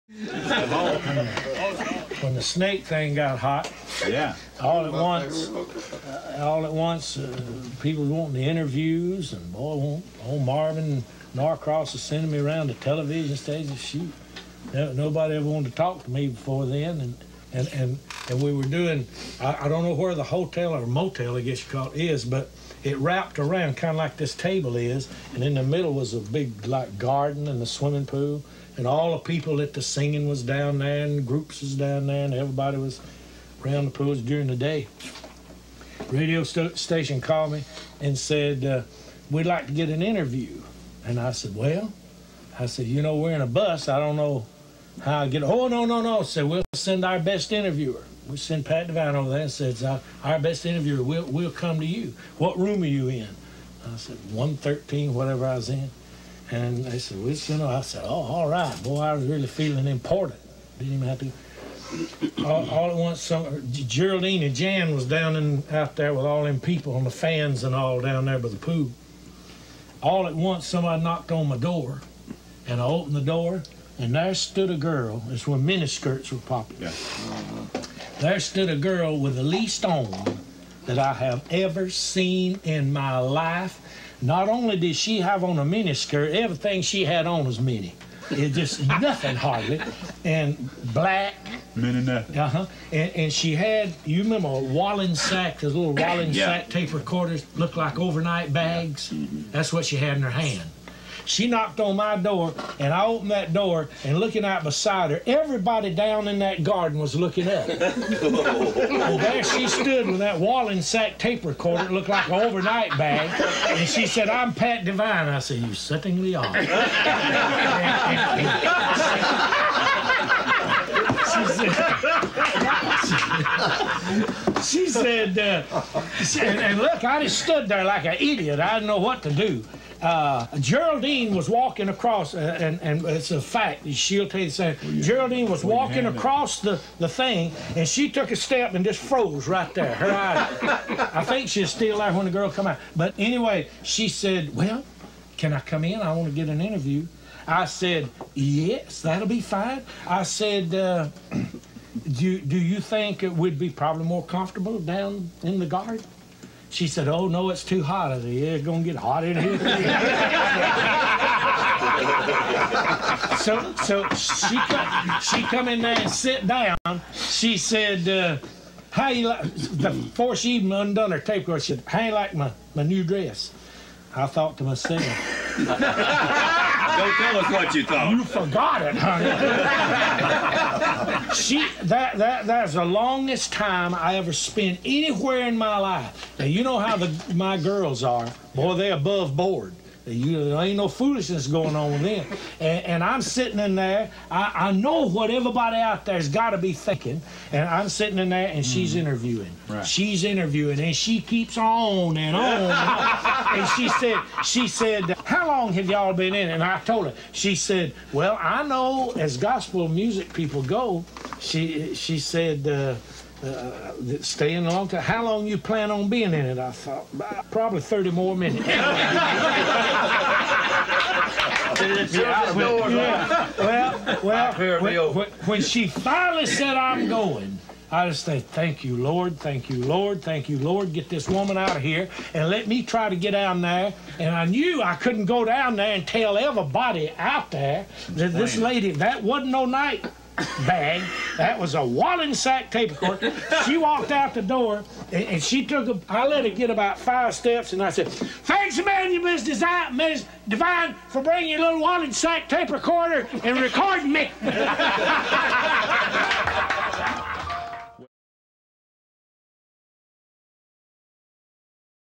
Southern Gospel Comedy - Wendy Bagwell - 'Wollensak Tape Recorder'
Here is some more Southern Gospel comedy from legendary Southern Gospel singer/songwriter …and master story-teller... Wendy Bagwell. Listen as Wendy recounts an uncomfortable interview moment that occurred on the heels of the mass popularity of his "Rattlesnake Story" success.